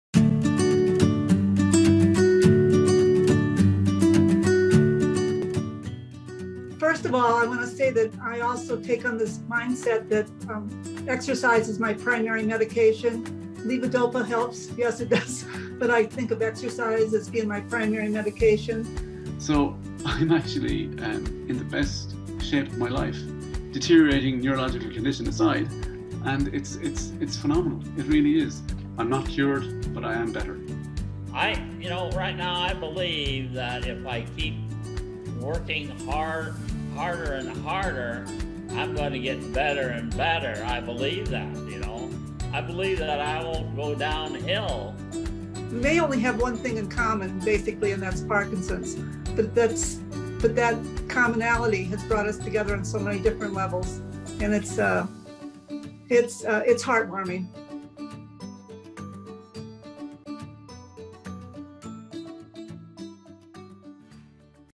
Parkinson's Awareness Month: LSVT Client Testimonial
Hear from Margie, Gary and Leon as they share a little of their stories in this Parkinson's Awareness Month: LSVT Client Testimonial.